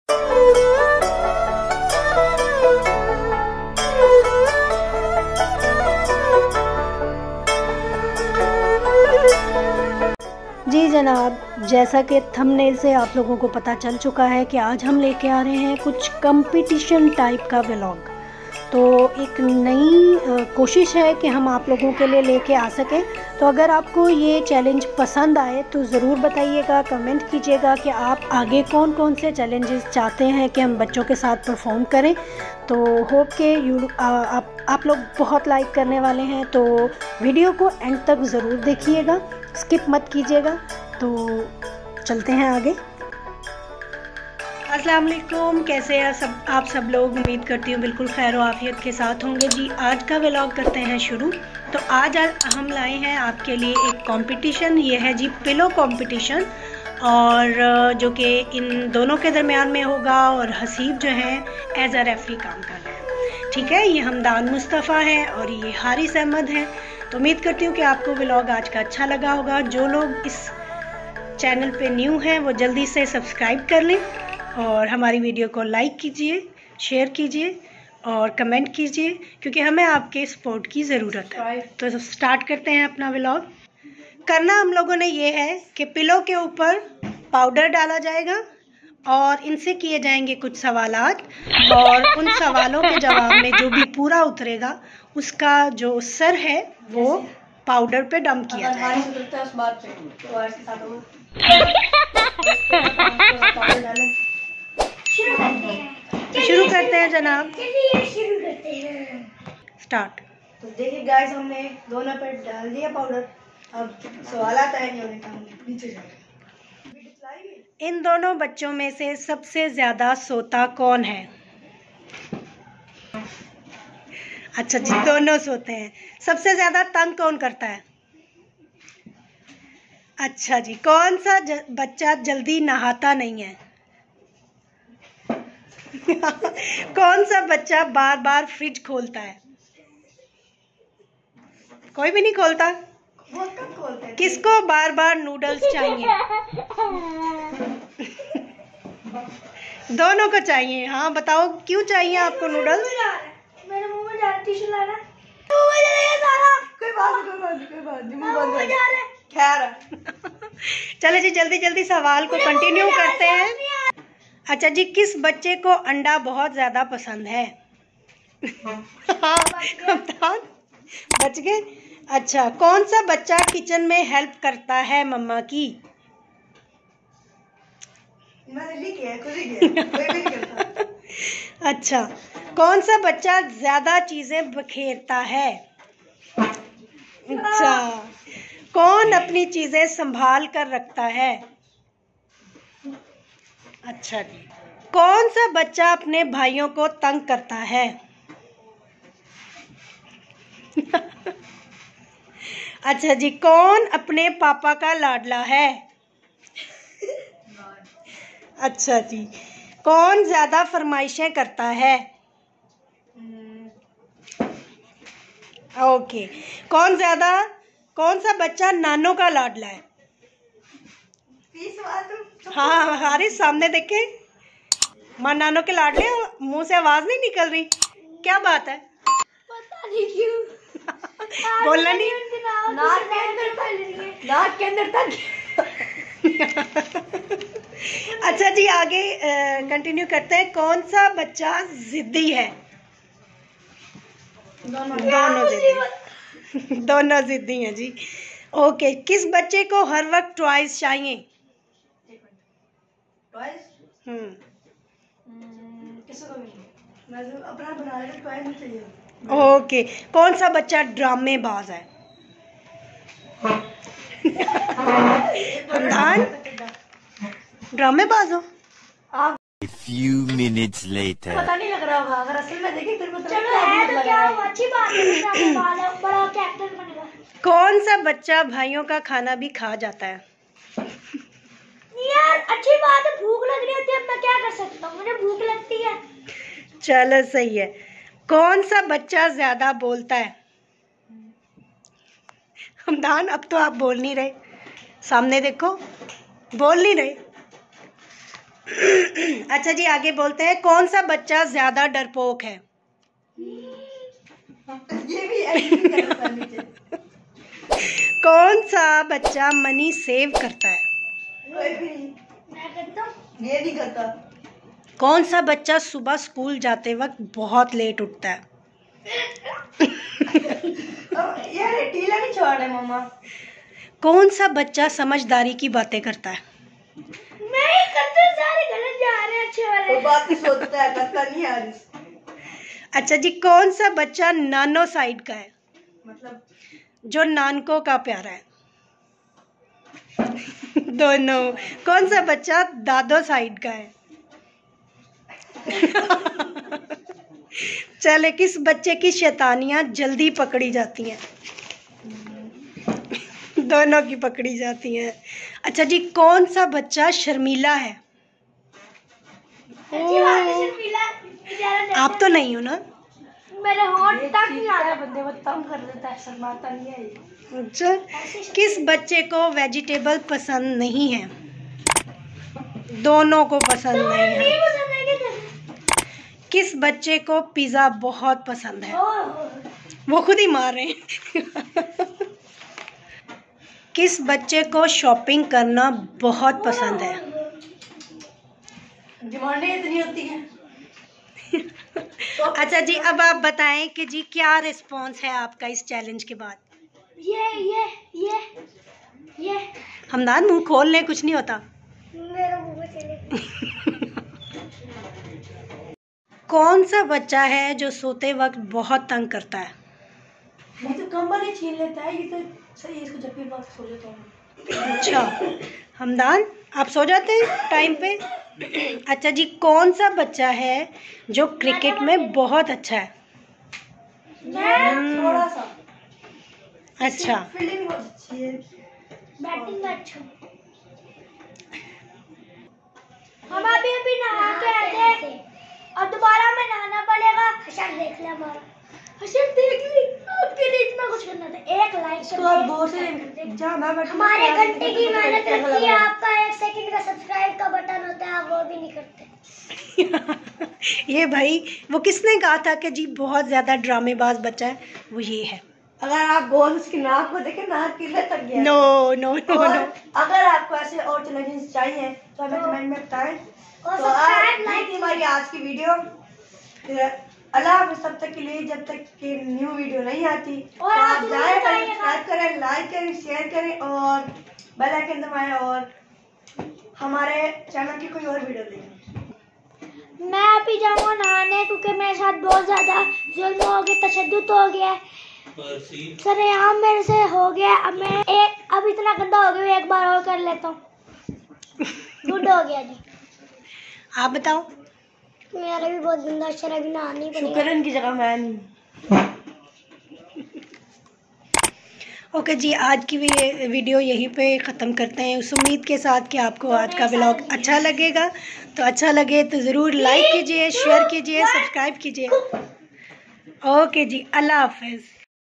Kids Pillow challenge vlog#59
Today we do kids pillow challenge, we all enjoy it alot . Hope you will also like it.